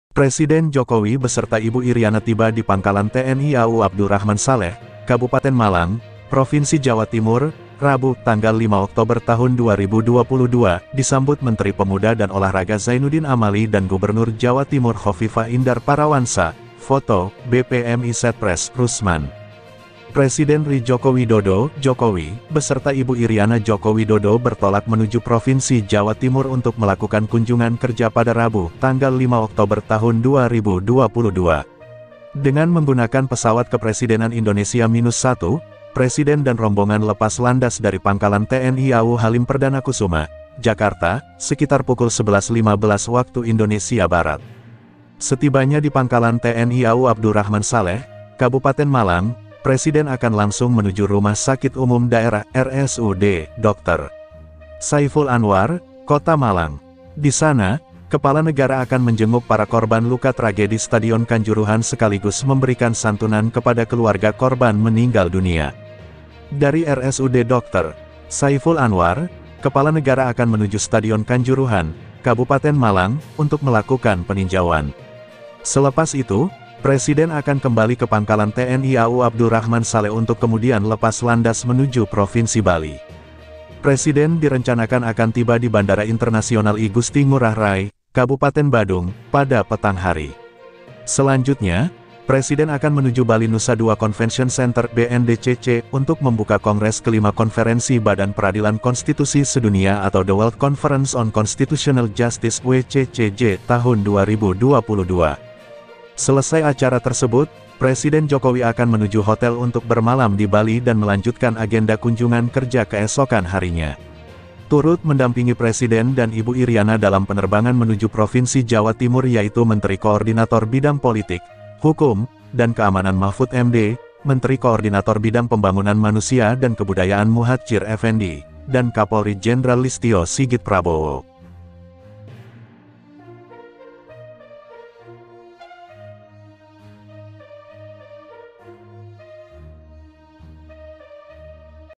Presiden Jokowi | Tinjau Stadion Kanjuruhan, Perbaiki Tata Kelola Persepakbolaan Indonesia
Tinjau Stadion Kanjuruhan, Presiden: Perbaiki Tata Kelola Persepakbolaan Indonesia